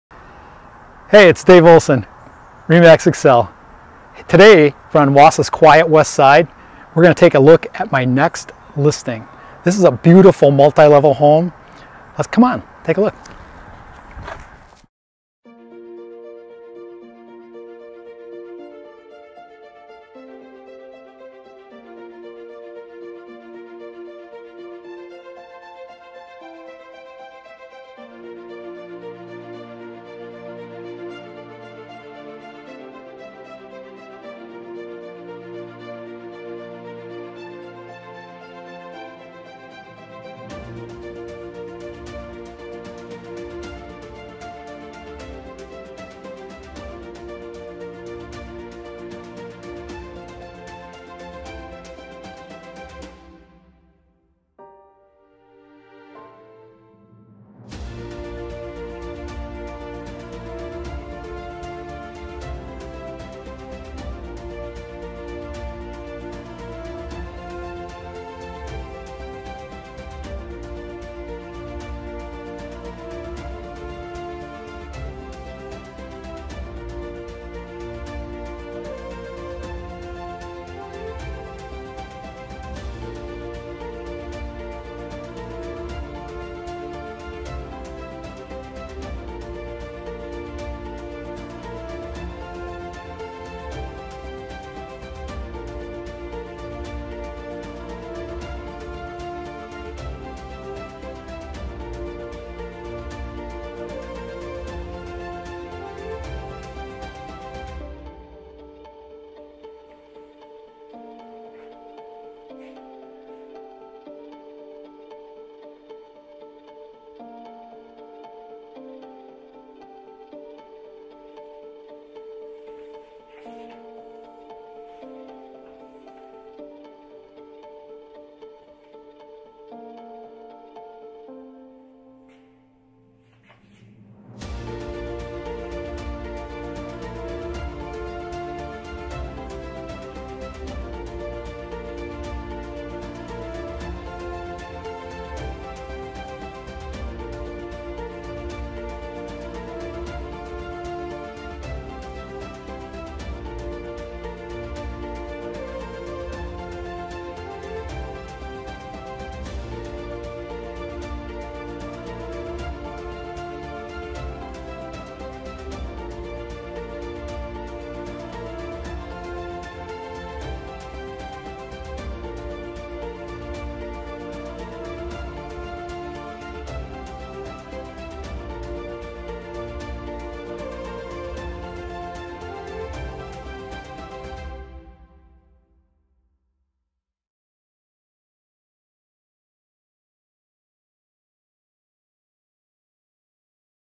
1230 ELM STREET, WAUSAU | $159,000
Well cared for West Side Tri-Level featuring a gorgeous updated custom kitchen with sloid cherry Cabinets, beautiful solid hardwood floors, granite countertops and fixtures with stainless steel appliances.  There is a sitting room, casual dining, or home office right off the kitchen or enjoy the massive wrap around deck for family gatherings.  Huge living room with fireplace and vaulted ceiling with wood beams.  Roomy bedrooms with spacious closets.  There is TONS of storage in this home with multiple storage rooms and closets conveniently placed throughout the home.  The lower level could easily become a private master suite with an inspired renovator – it is already started for you with new laminate floors and a ceiling with recessed lighting!  Incredible quiet West Side location that is close to shopping, parks, and schools with easy highway access.